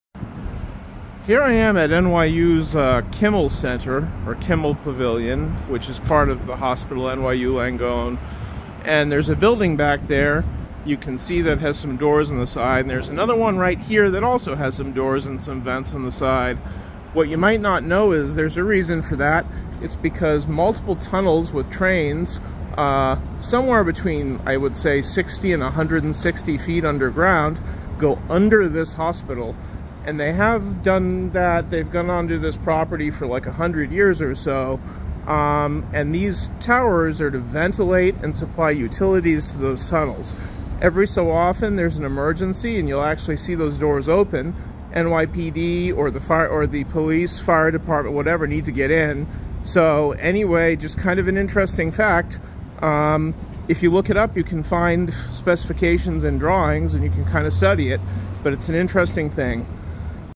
UNDER YOUR FEET - Amtrak and LIRR (East Side Tubes Ventilators)
Over 100 feet under the Kimmel Pavillion of NYU-Langone Hospital (on the far east side of Manhattan) run several tunnels carrying Amtrak and LIRR trains under the East River to and from Queens.

Sharing physical land with the hospital are two large, oddly-shaped buildings. these bring fresh air into the tunnels as well as provide access to and from the tunnel for maintenance, emergency exit, etc.